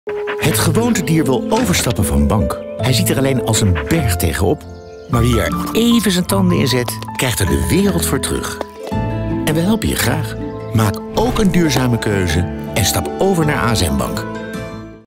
Het Gewoontedier stapt over
Het Gewoontedier wil overstappen naar een duurzame bank. Hij ziet er alleen als een berg tegenop. En eerlijk is eerlijk: overstappen kost ook even moeite. Daarom helpen we je erbij! Kijk voor meer informatie op